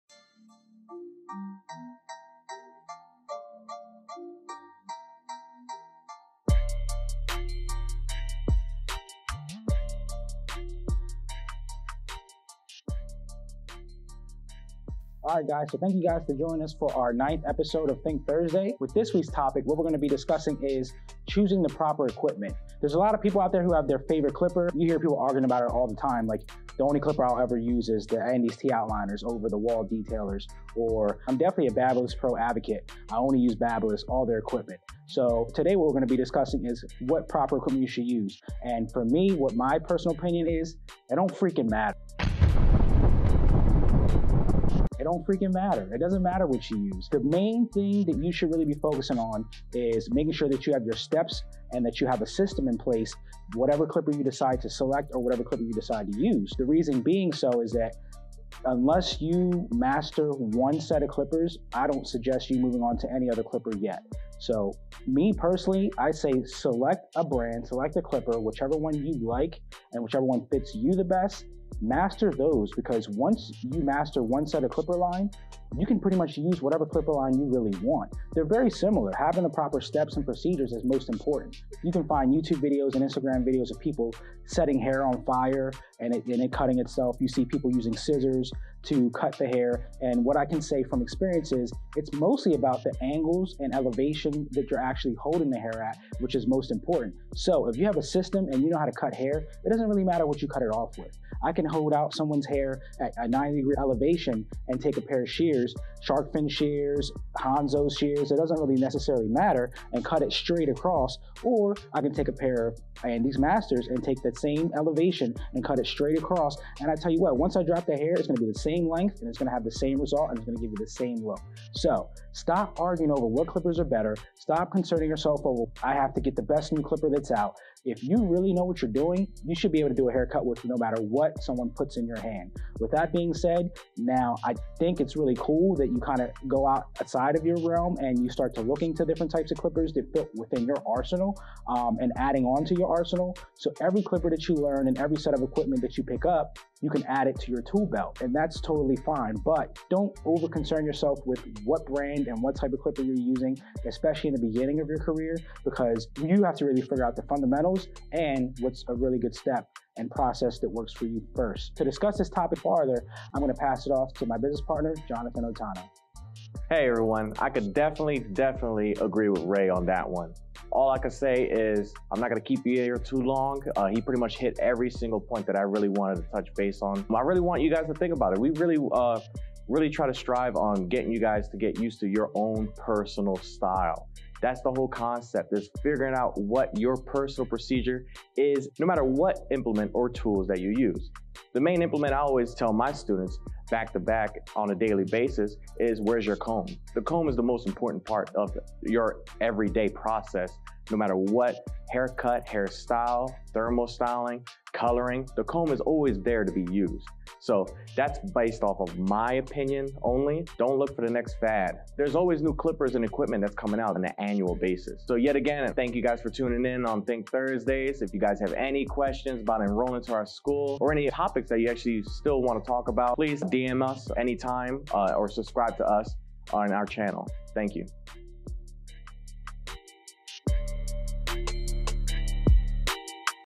EPISODE 9- CHOOSING THE PROPER EQUIPMENT
Don't know what clippers you should buy? Watch as Master Barber Instructors Ray Noel and Jonathan Otano explain their opinion on the best clippers to use.